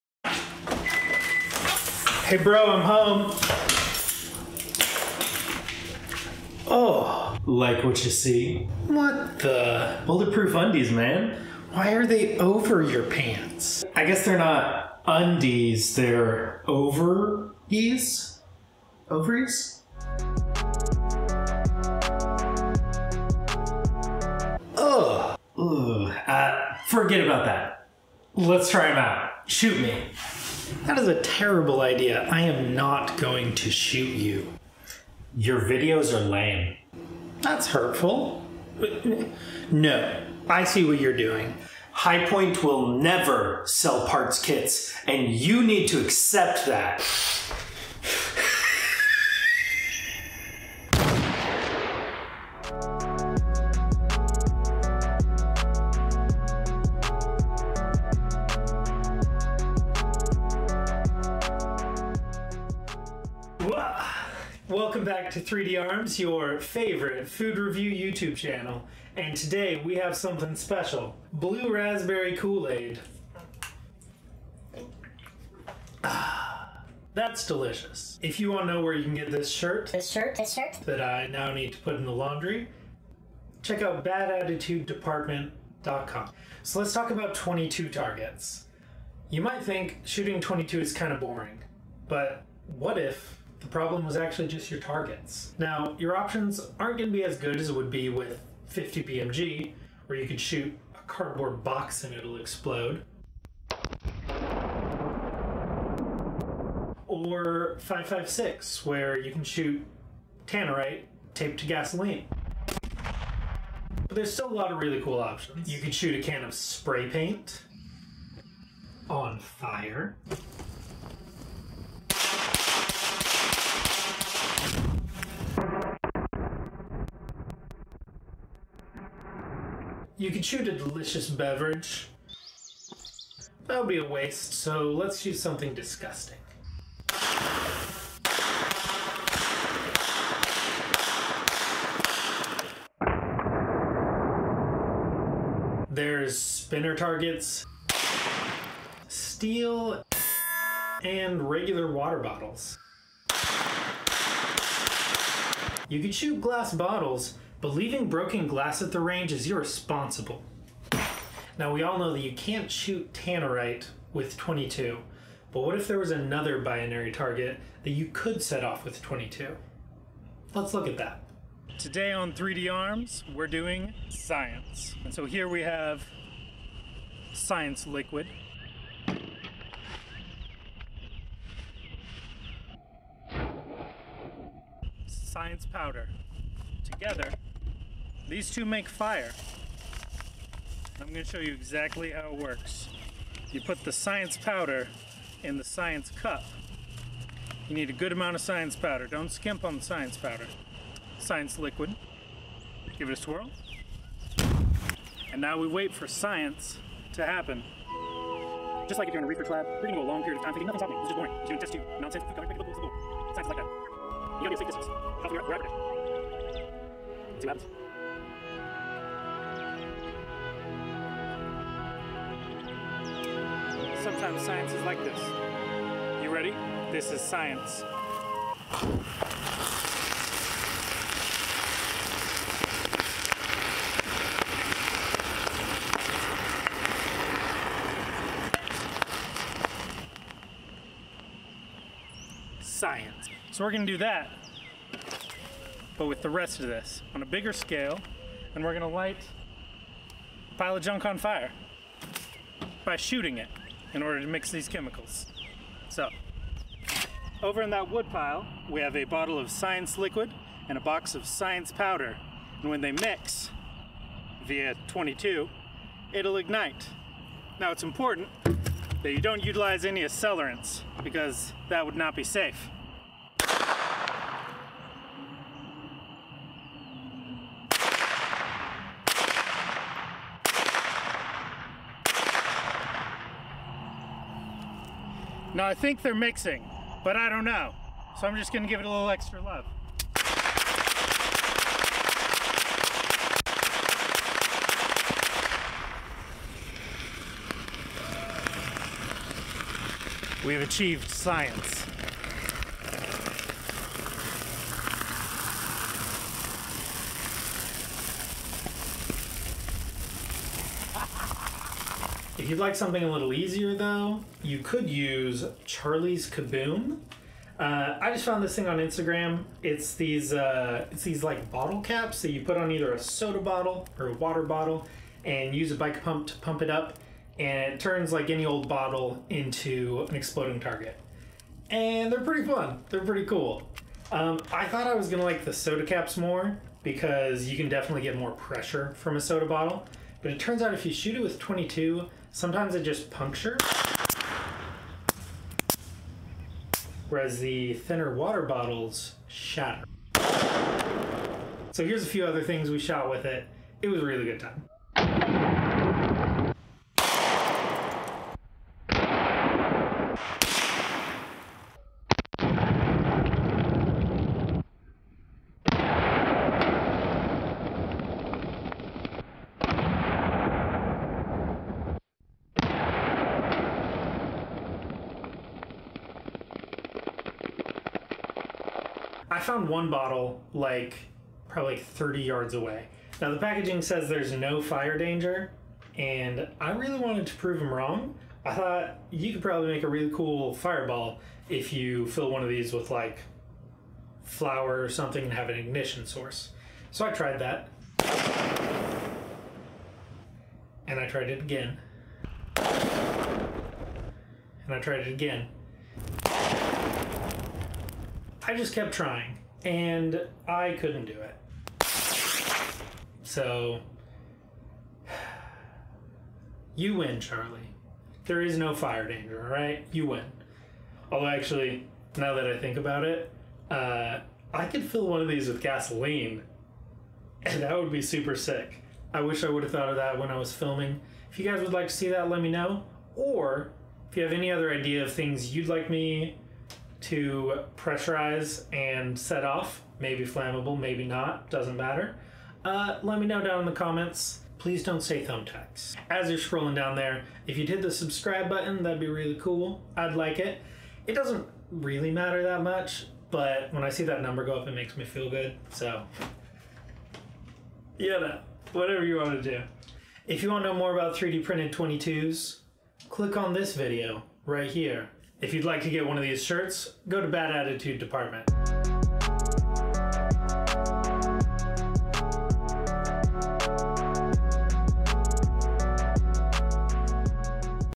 Targets for 22LR; Explosions, Infernos, and the Second Best Reason to Be a Chemist
The bottle caps (no gunparts, so don't strike my channel youtube)